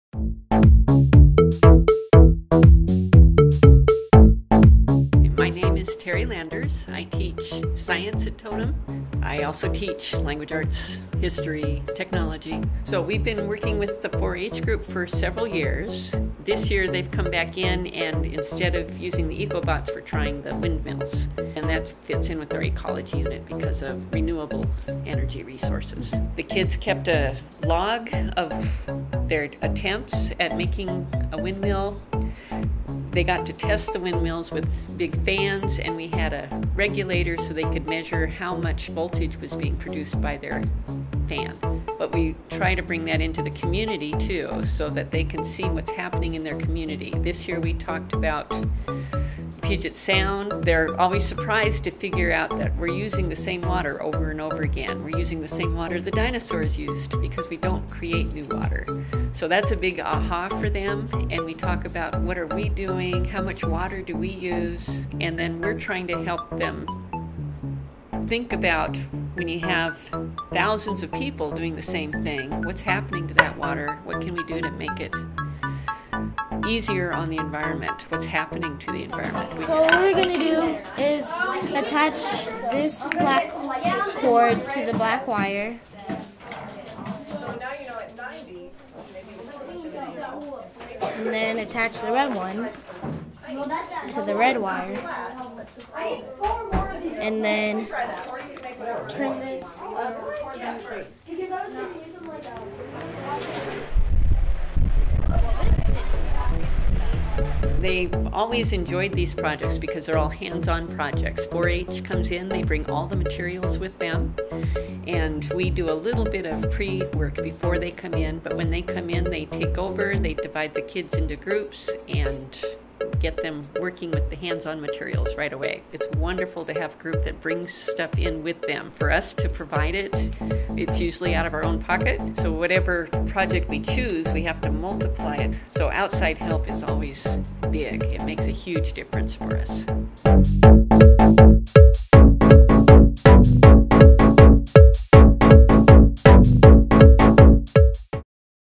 Totem Middle Schools Harnesses the Wind
This winter students at Totem Middle School had a visit from 4H for tutorials on building their own windmills as part of their Ecology unit. Check out this video of Ms. Landers' class building and testing their windmills.